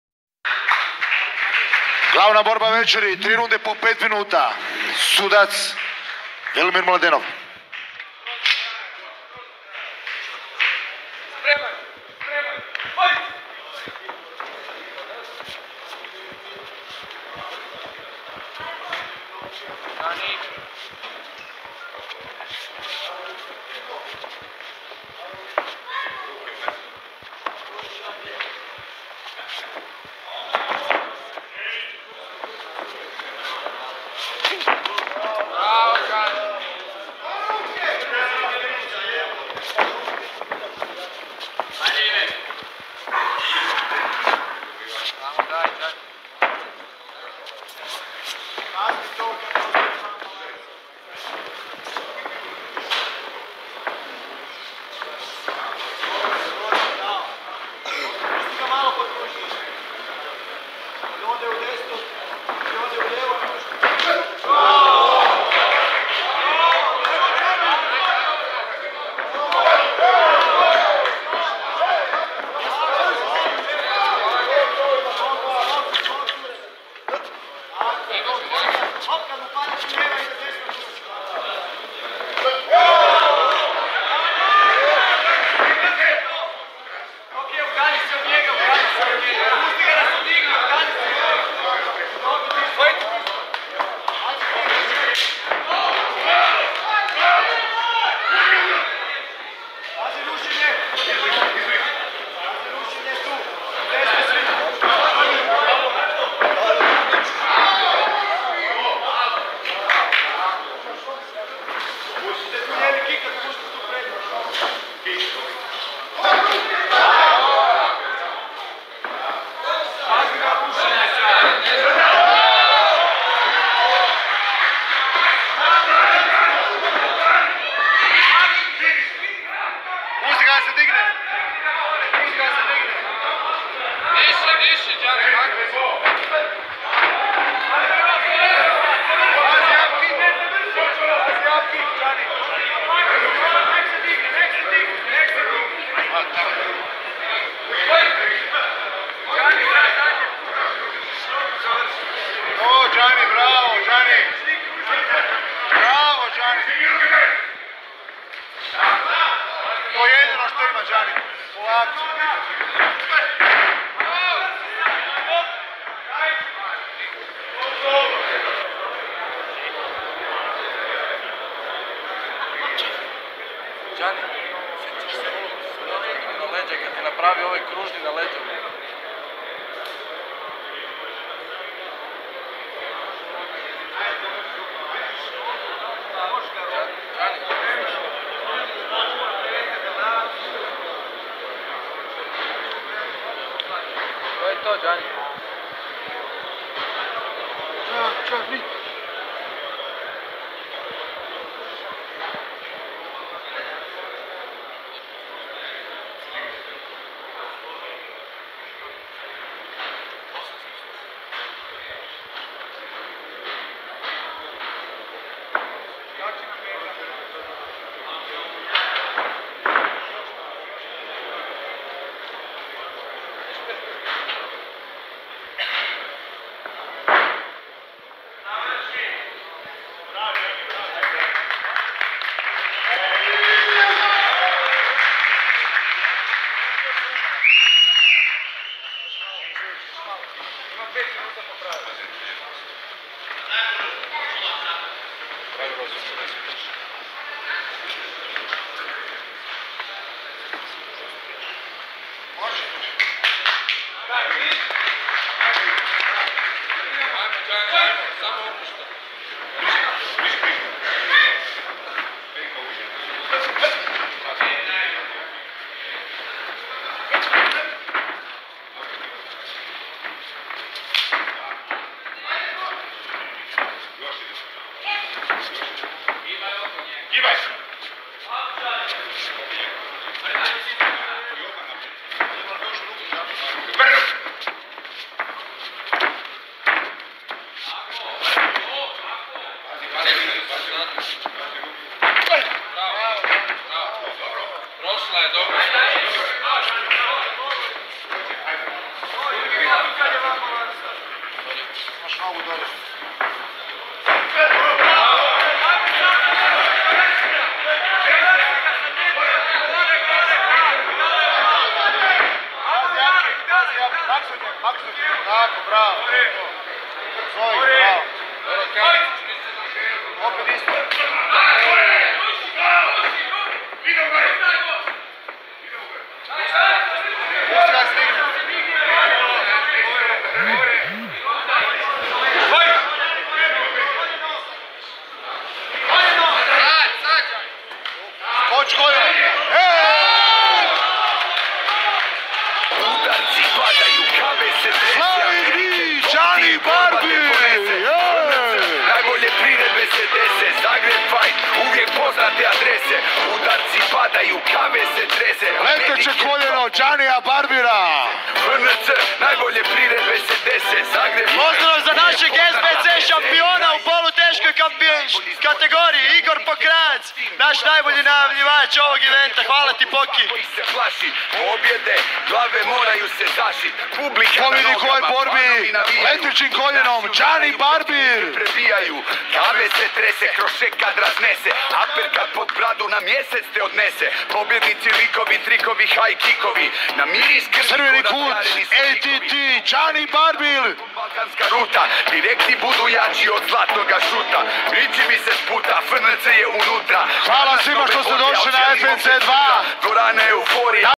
FNC 2 | Đani Barbir VS Ivan Karačić
FNC 2 odrzan u Zagrebu u dvorani ATT Zagreb. U glavnoj borbi veceri sastali su se Đani Barbir i Ivan Karačić. Borba je odradjena u kategoriji do 84kg.